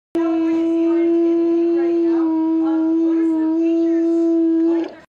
Chase's Song
“OoooooOooOoo” by Chase the sea lion 🎶🎤🎵

Video by Robin Riggs